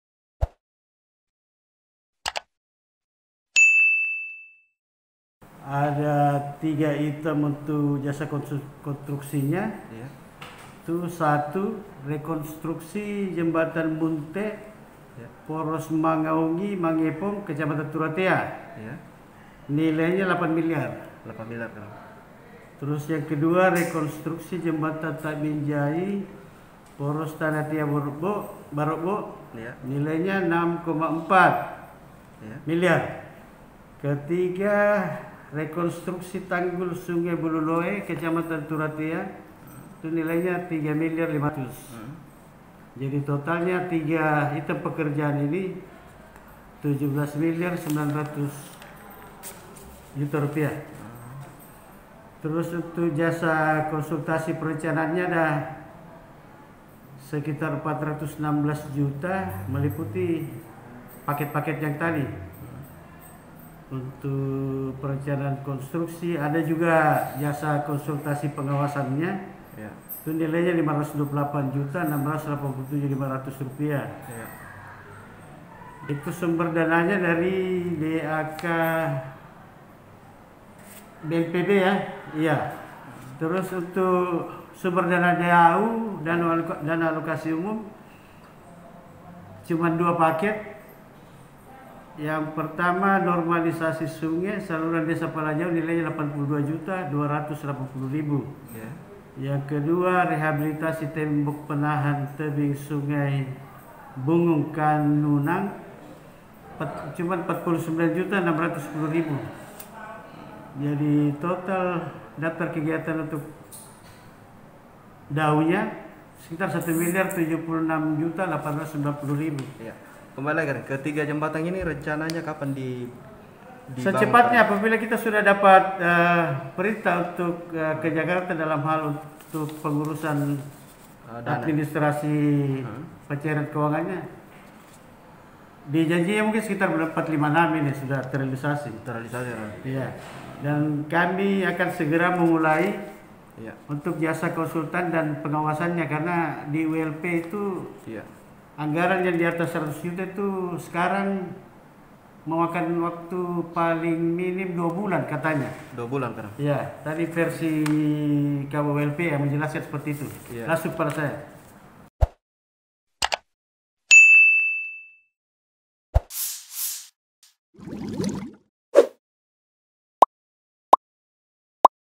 Tahun Ini, BPBD Jeneponto Bangun Jembatan Munte dan Ta’binjai
Badan Penanggulangan Bencana Daerah (BPBD) Jeneponto rencan akan membangun kembali dua jembatan dan satu tanggul sungai yang rusak berat akibat dampak banjir bandang pada Januari 2019 lalu.

Rencana pembangunannya sendiri dilakukan tahun ini dengan sumber anggaran dari dana hibah Anggaran Pendapatan dan Belanja Negara (ABPN) di l Badan Nasional Penanggulangan Bencana (BNPB).

Hal tersebut disampaikan PLT Kepala BPBD Jeneponto, Ikrar Iskandar kepada Sulselsatu.com saat ditemui di ruang kerjanya, Senin (22/3/2021).

“Kedua jembatan yang akan rencana dibangun tahun ini yakni Jembatan Munte dan Ta’binjai di Kecamatan Turatea. Sementara untuk pembangunan tanggul sungai di Bululoe. Semuanya angarannya dari ABPN,” ujar Ikrar.

Untuk pembangunan jembatan Munte nilai anggaranya kata Ikrar, Rp8 miliar, rekonstruksi Jembatan Ta’binjai di poros Tanetea Baro’bo, anggarannya Rp6,4 miliar dan rekonstruksi pembangunan tanggul Sungai Bulu Loe anggaranya Rp3,5 Miliar.

Sementara jasa konsultasi perencanaan ke tiga item kegiatan tersebut, sebesar Rp416 juta.

Karaeng Kulle sapaannya mengaku, jadwal pembangunannya jika tak kena refocusing anggaran, dibangun antara bulan April atau Juni tahun ini.

“Secepatnya (pembangunan) apabila kita sudah dapat perintah untuk ke Jakarta dalam pengurusan administrasi pencairan keuangannya. Di janjinya sekitar bulan 4 dan 6 sudah teralisasi,” jelas Ikrar.

Video terkini dan terbaru..